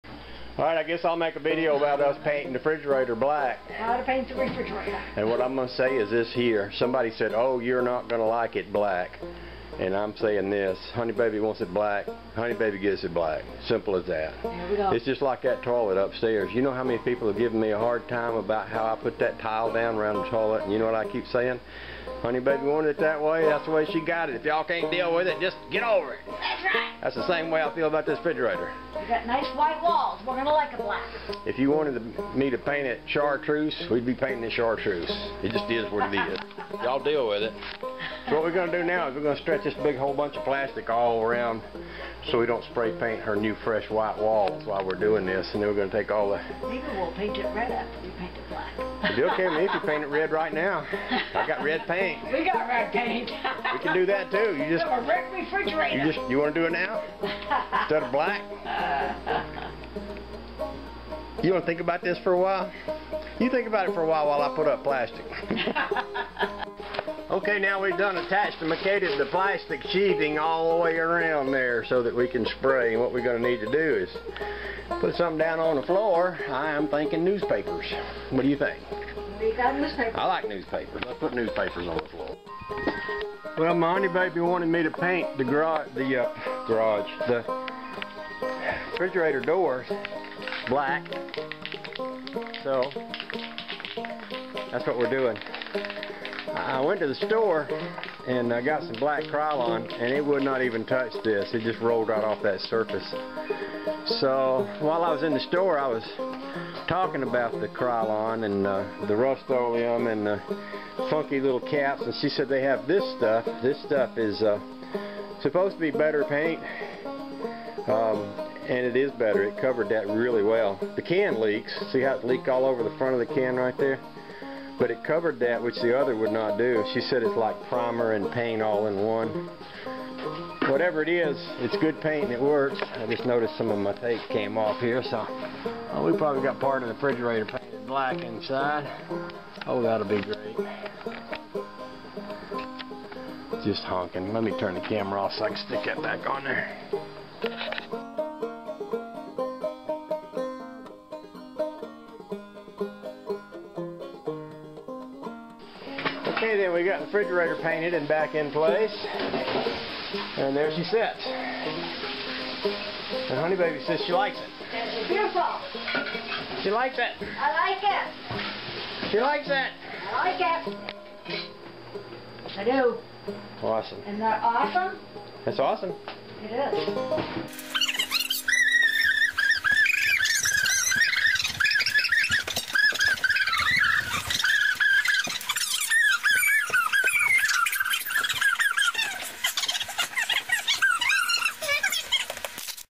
Painting the Fridge
and a happy dance 'cause it looks so cool! Rustoleum Paint

Rustoleum spray paint

Rustoleum Latex Exterior High Gloss paint here: